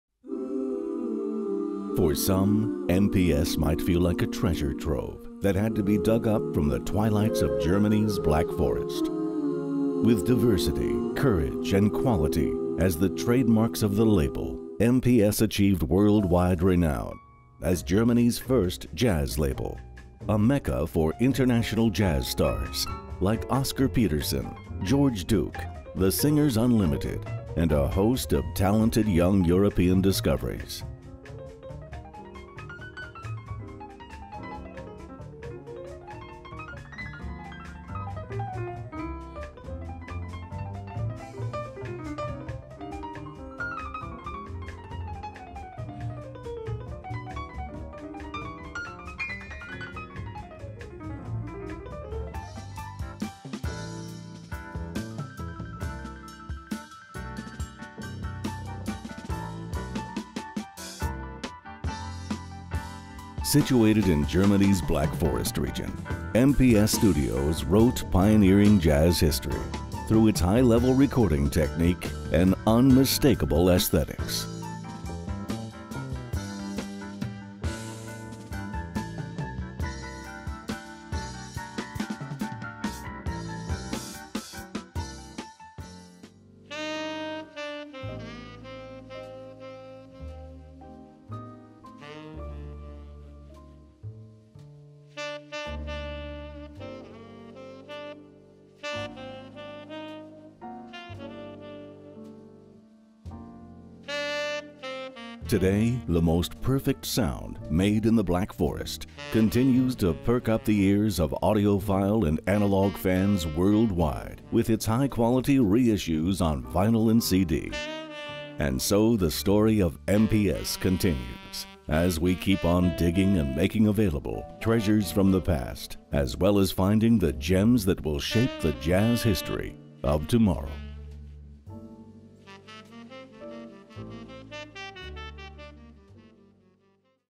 MPS - The German Jazz Icon (Musik Produktion Schwarzwald)
For some MPS might feel like a treasure trove that had to be dug up from the twilights of Germany’s Black Forest. With diversity, courage, and quality as the trademarks of the label, founder Hans Georg Brunner-Schwer achieved world-wide renown as Germany’s first jazz label. A Mecca for international jazz stars like Oscar Peterson, George Duke, The Singers Unlimited and a host of talented young European discoveries. Situated in Villingen, in Germany’s Black Forest region, for some two decades MPS Records and studios wrote pioneering jazz history through its high-level recording technique and unmistakable aesthetic. Today the “most perfect sound made in the Black Forest” continues to light up the ears of analogue fans worldwide.